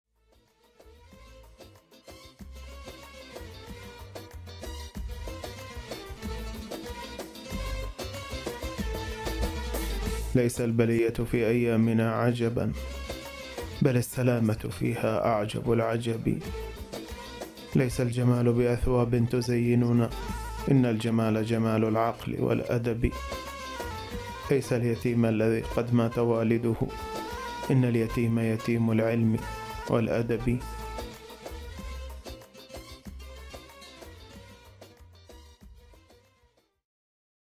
ليس اليتيم الذي قد مات والده
آبيات من ديوان الإمام علي عليه السلام (مع خلفية موسيقية)